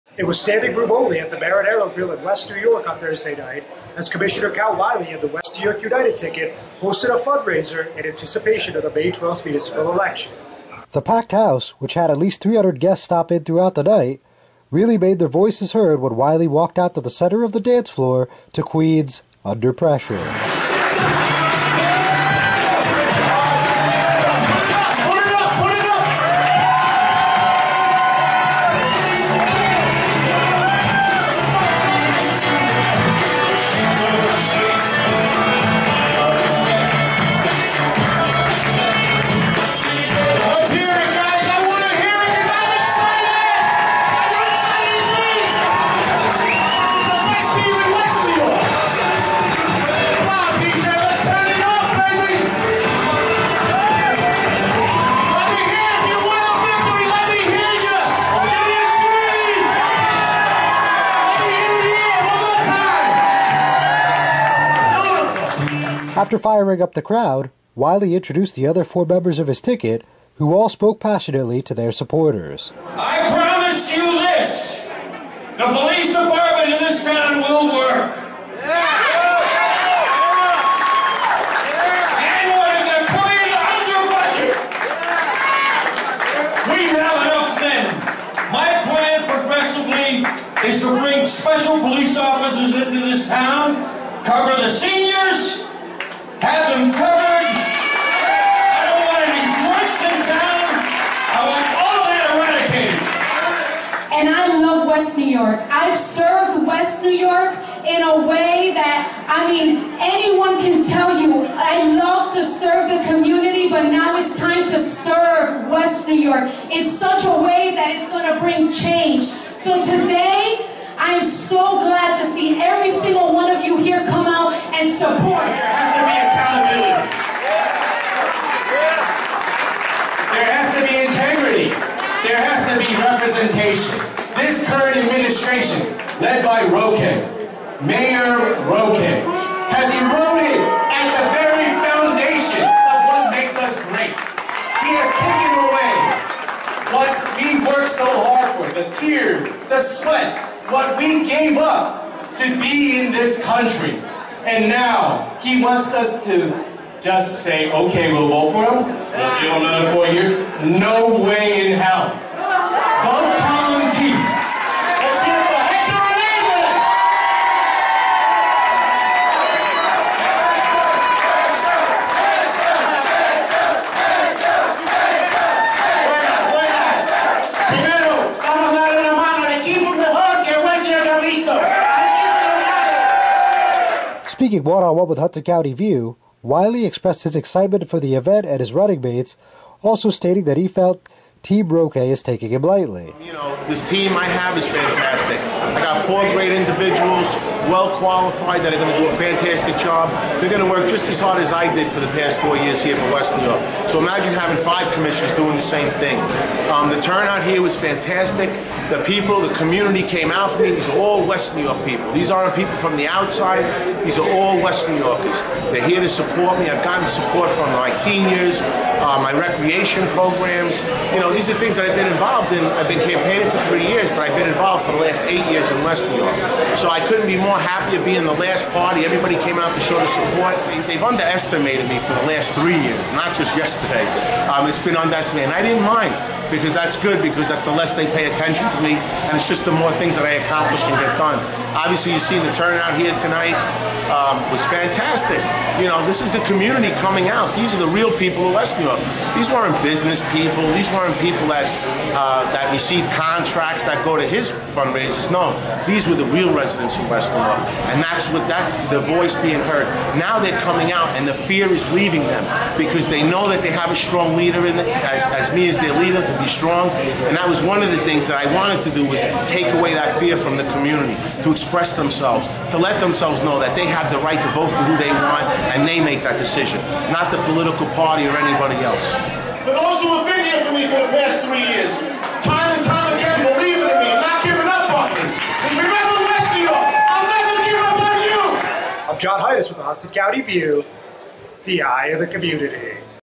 Count Wiley's ticket brings huge crowd to Marinero Grill
Led by Commissioner Count Wiley, the West New York United ticket hosted a packed event at the Marinero Grill last night where they assured victory against Mayor Felix Roque and his slate in the May 12 municipal election.